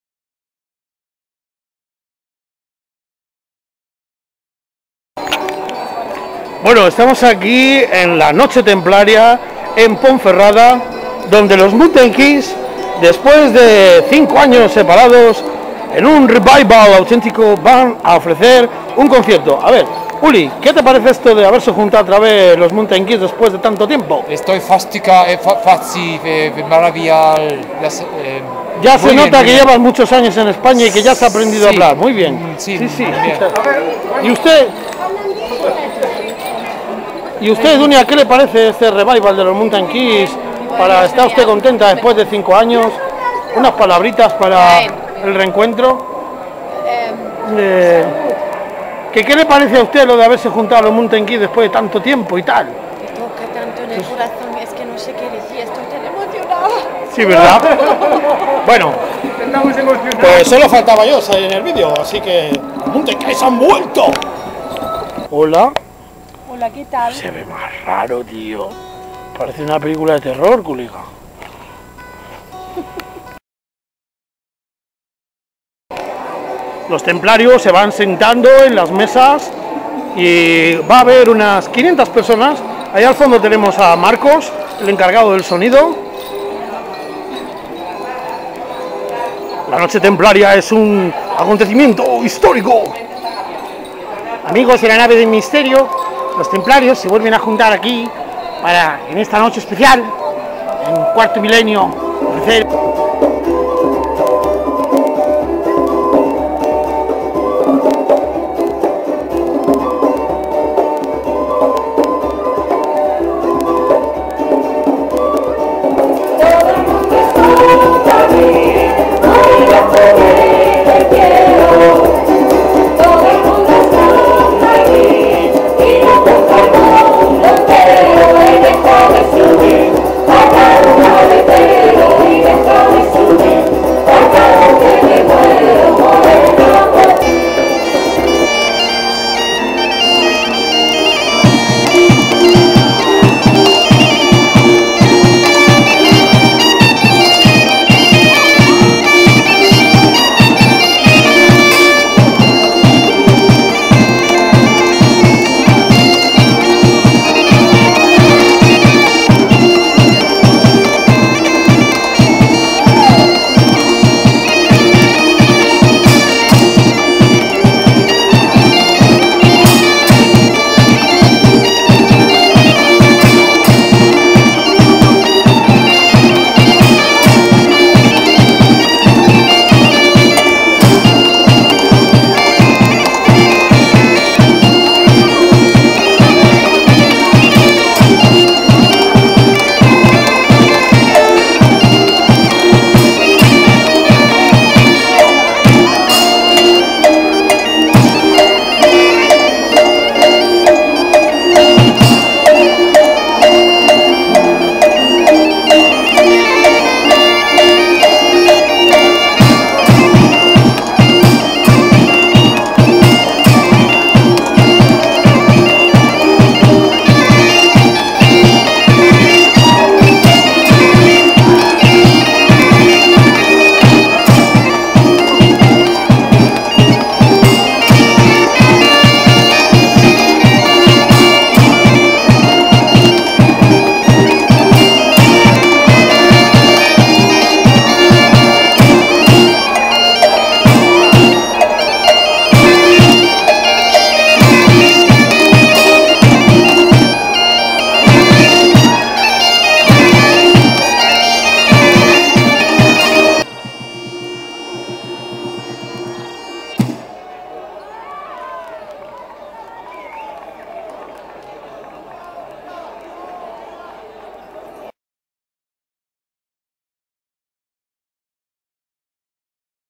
mountain kids en cena templaria en ponferrada
en el castillo nos volvimos a juntar despues de tres años los mountain kids